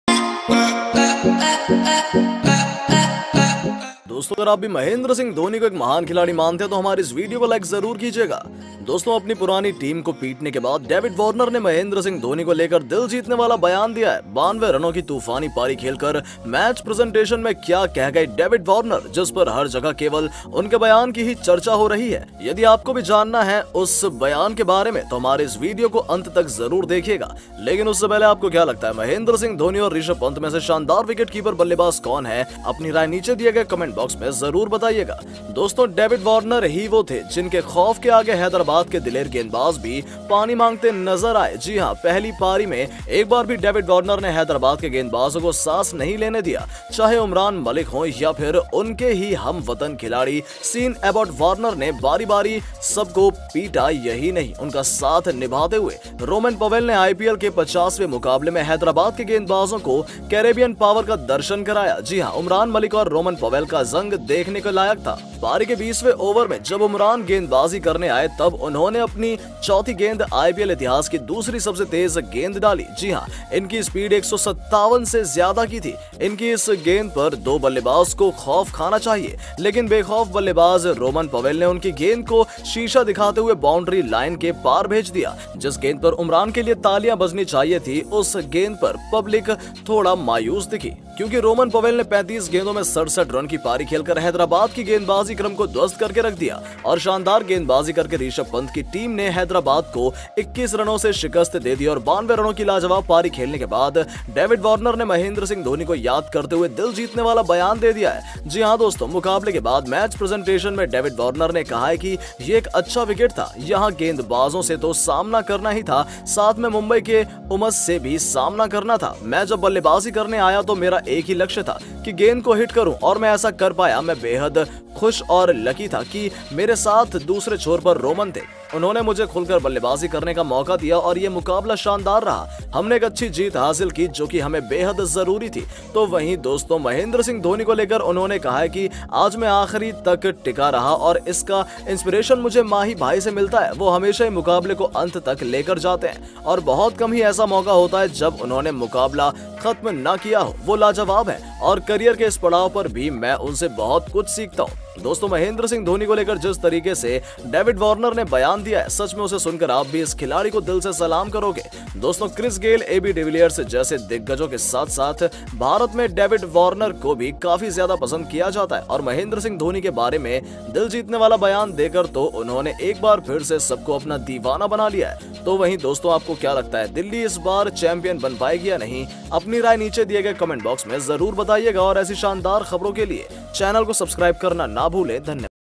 देखिए, SRH के खिलाफ जीत के बाद Dhoni को भगवान बताकर रो पडे Warner ऐसा कहकर जीता करोडो भारतीयो का दिल
Delhi capitals vs Sunrisers Hyderabad Full Match Highlights , DC VS SRH FULL HIGHLIGHT Pant Dhawan